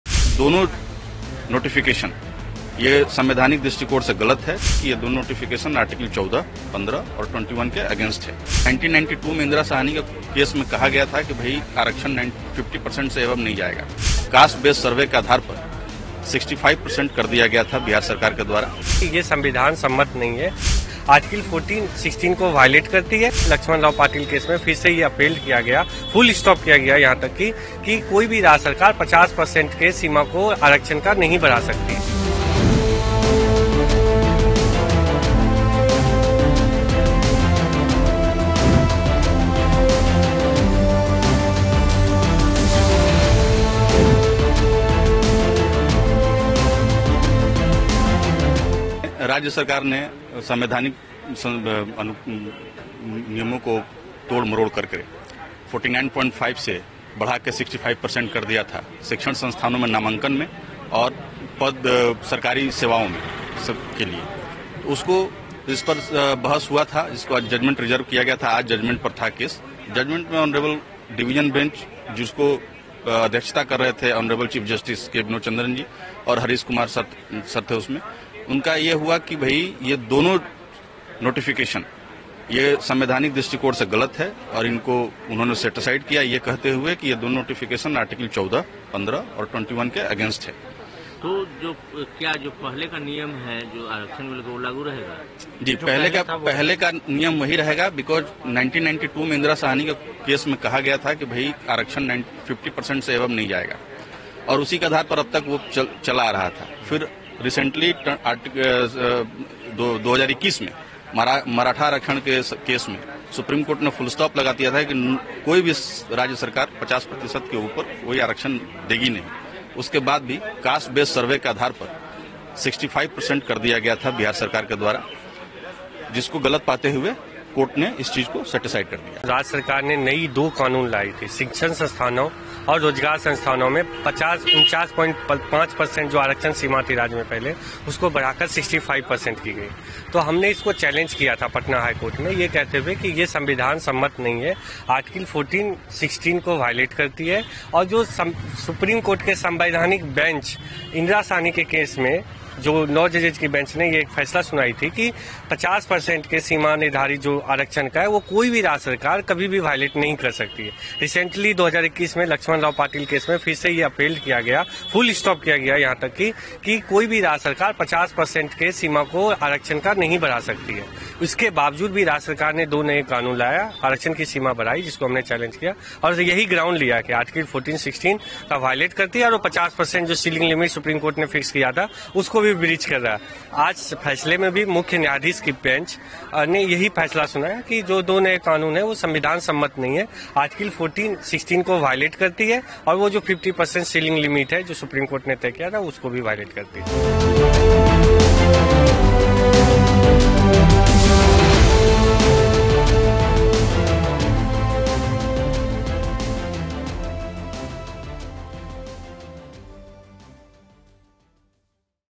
Patna HC scraps Bihar’s decision to increase quota for backward classes to 65 pc
The Patna High Court has overturned the Bihar government's decision to raise the quota for backward classes to 65 percent. In its ruling, the court stated that this increase violated Articles 14, 15, and 16 of the Constitution and exceeded constitutional limits. The revised reservation scheme allocated 20 percent for Scheduled Castes, 2 percent for Scheduled Tribes, and 43 percent for Other Backward Classes and Extremely Backward Classes, reducing the open merit category to 35 percent.